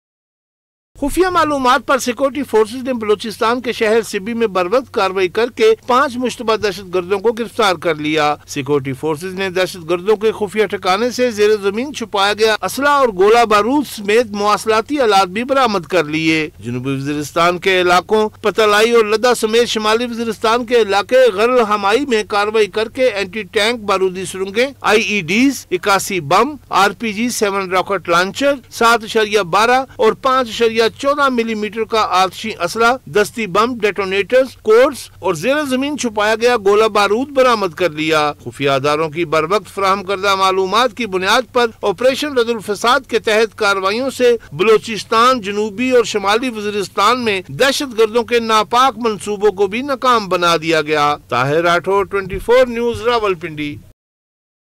ملک میں دہشتگردی اور تباہی کے بڑے منصوبے نا کام بنا دئیےگئے
24 News HD is one of the leading news channels of Pakistan bringing you the latest current affairs from Pakistan and around the world.